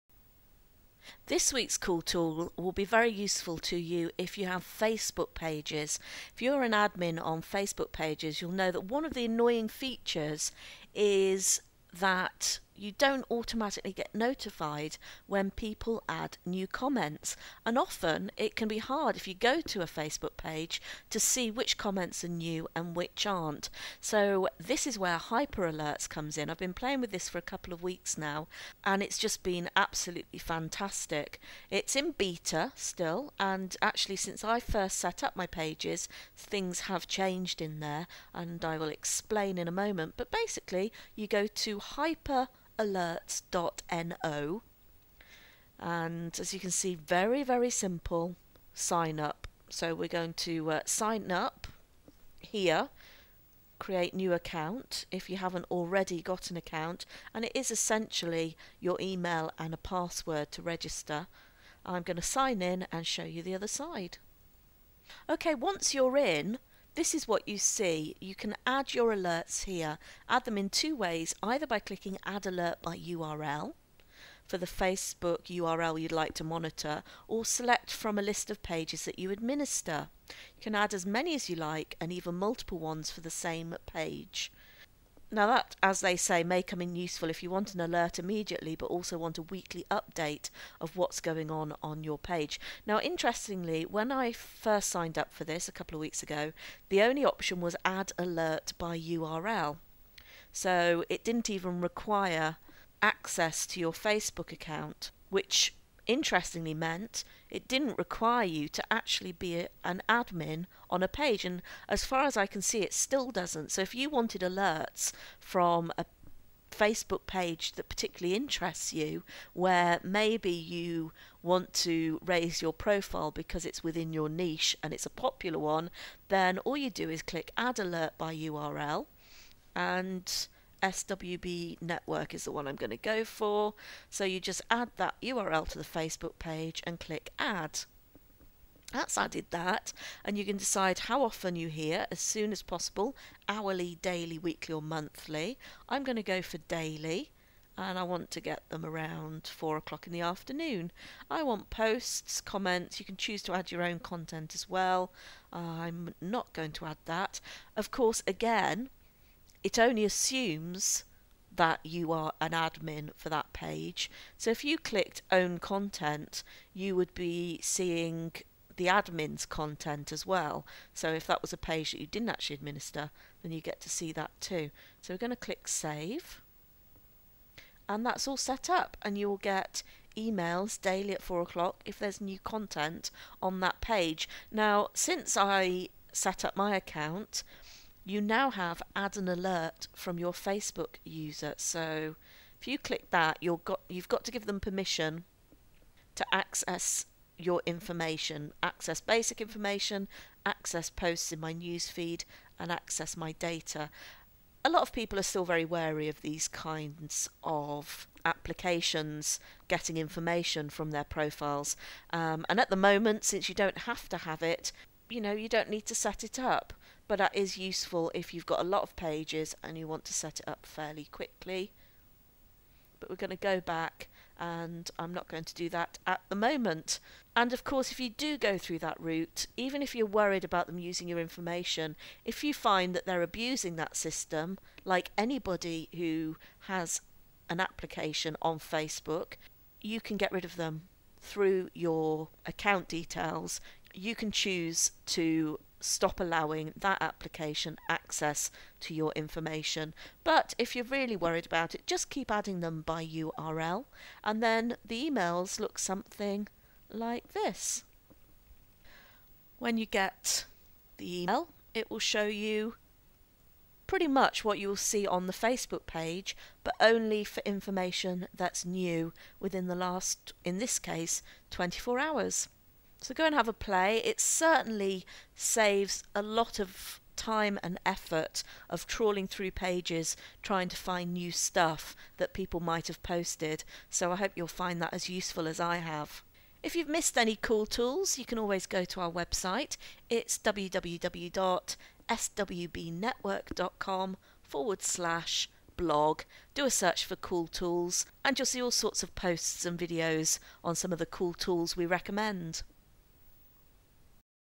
Hyper Alerts: Monitor New Facebook Pages Comments
This week's "Cool Tool" is a new kid on the Facebook block and is still in Beta. Hyper Alerts solves one of the main problems Facebook Pages admins face - how do you know when new comments are posted on your page without visiting them constantly (and having to scan through every wall post)?

With Hyper Alerts you can set it up to deliver regular alerts to your email of new comments that have been posted to your Facebook pages.

hyper alerts
monitor facebook pages
facebook pages notifications